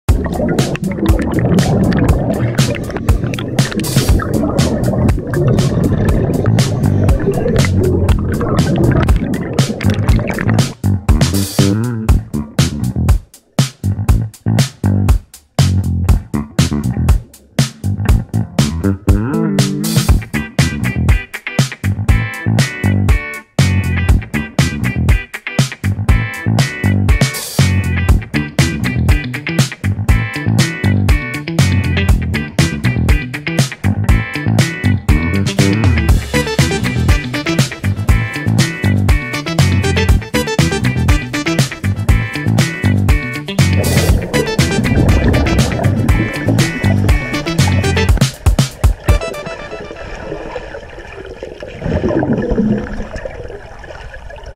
Daniel Gigante Discover Scuba Diving
Daniel Discovers the underwater world for his birthday, next time invite all your friends for an underwater adventure...